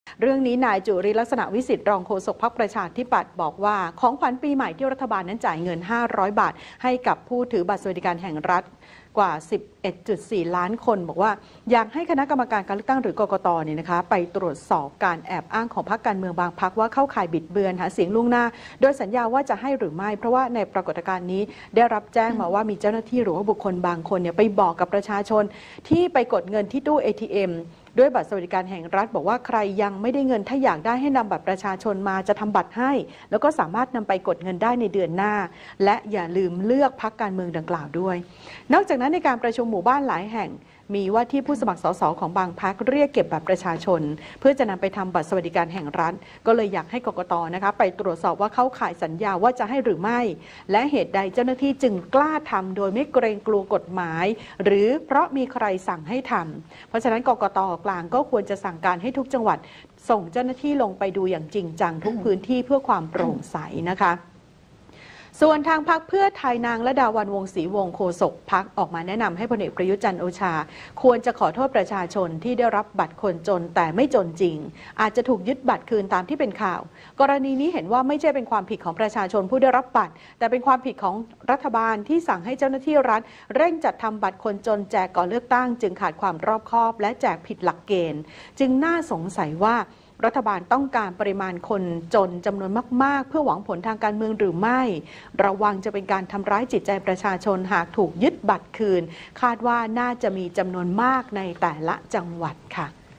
พท.-ปชป.ประสานเสียงถล่มรัฐบาล แจกเงินคนจน
2018/12/12 พท.-ปชป.ประสานเสียงถล่มรัฐบาล แจกเงินคนจน